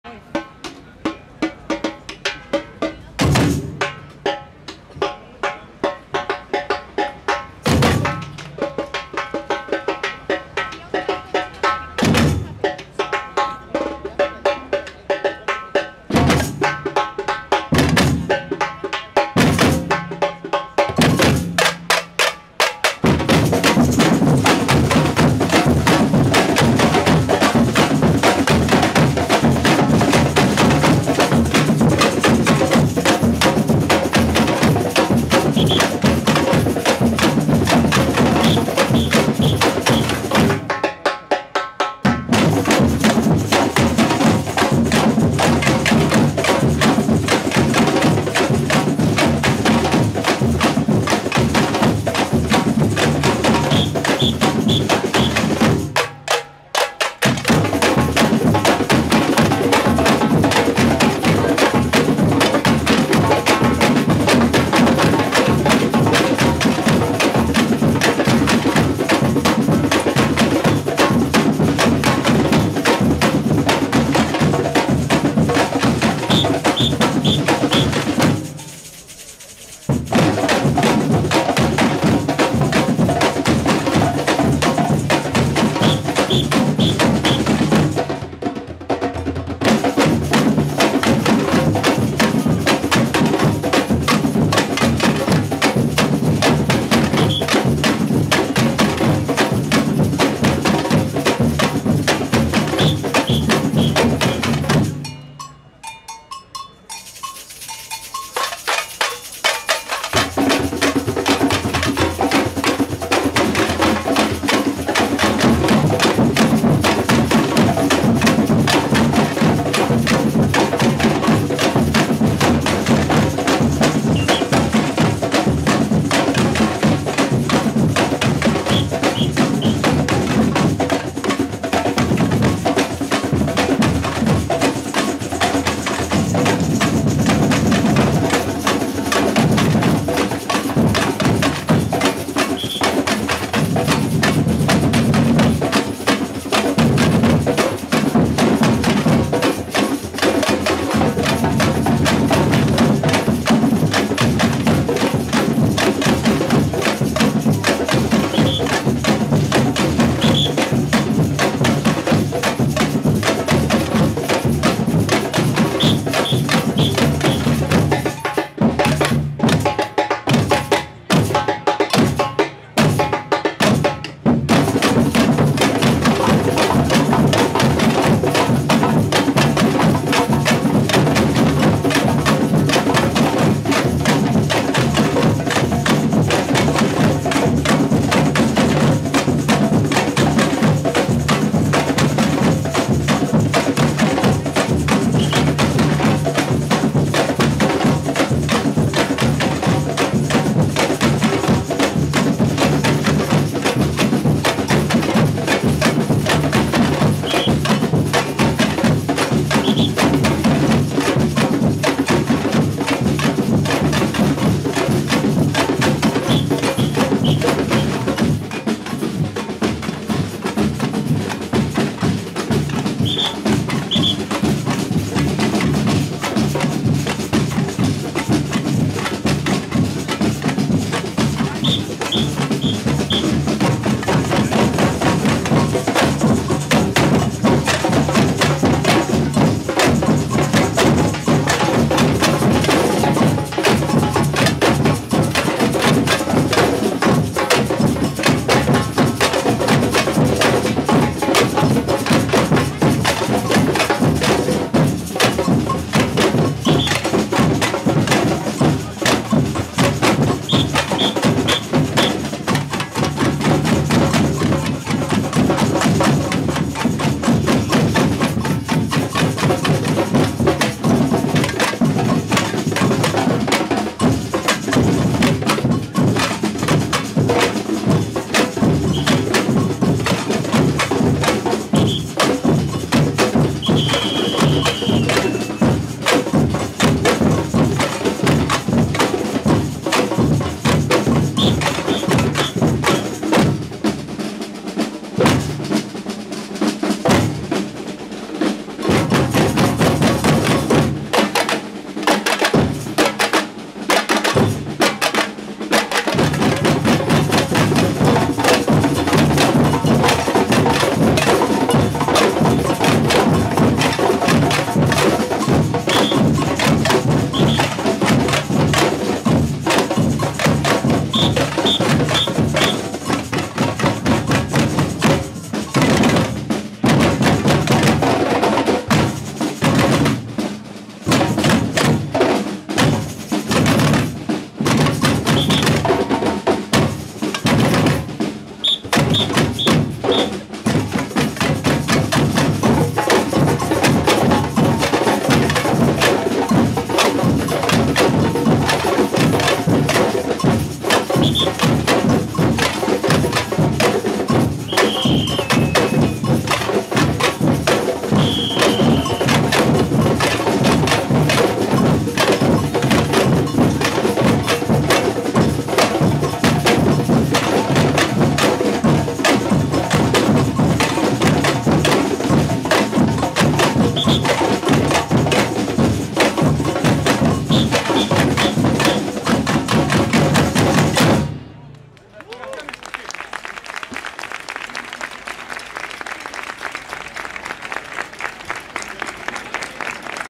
X aniversario Jose Couso - Samba da Rua
Samba da Rua cierra el homenaje a Jose Couso ante la embajada de Estados Unidos en Madrid. en el 10º aniversario de su asesinato en el hotel Palestina de Bagdad el 8 de Abril de 2003,  atacado por las tropas de Estados Unidos, en un intento de callar a la prensa libre que cubría la invasión de Irak.